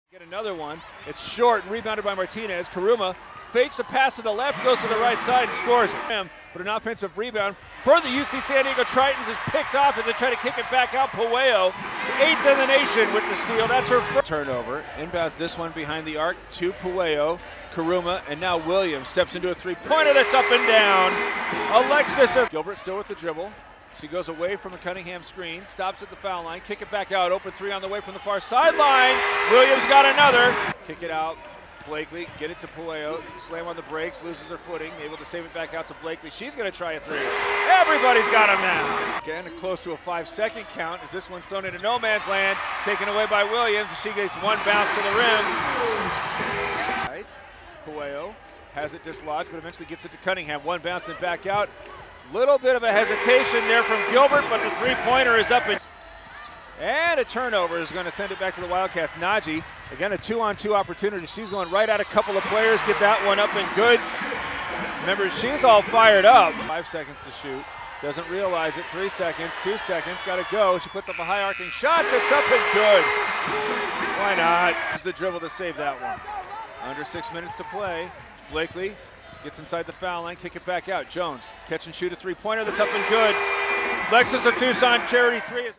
Wildcats Crush the Tritons
Arizona buries UC San Diego under a barrage of three's.